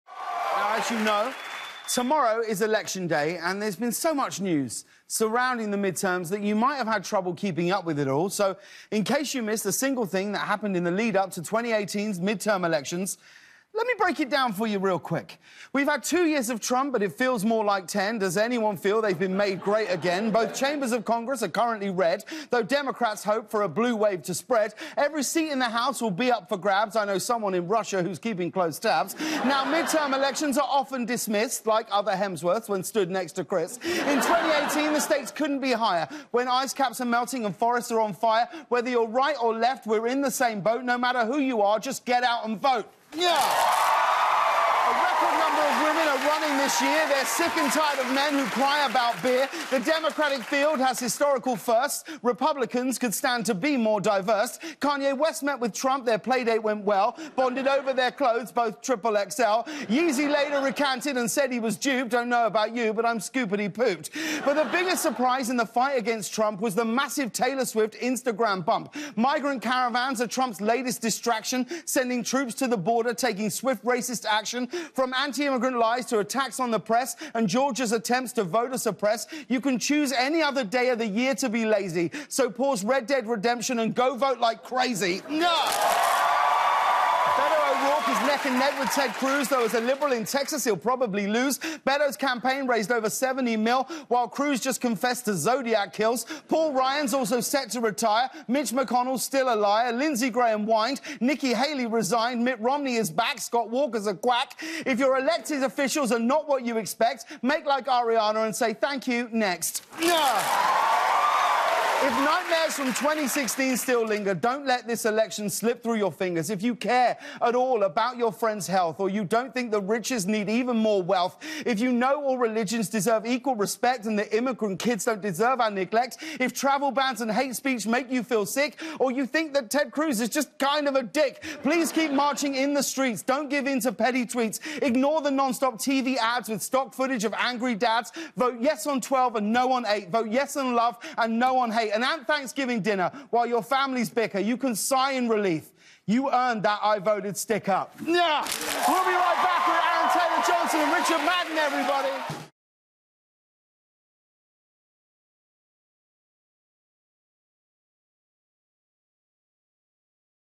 From Taylor to Ted: James Breaks Down the '18 Election
On the eve of the 2018 midterm elections, James recaps the madness of the last 2 years, beginning with President Donald Trump taking office. From Kanye West's MAGA hat to Taylor Swift's impact  Tennessee's vote for Senator, all of it has led up to November 6. Go out and vote!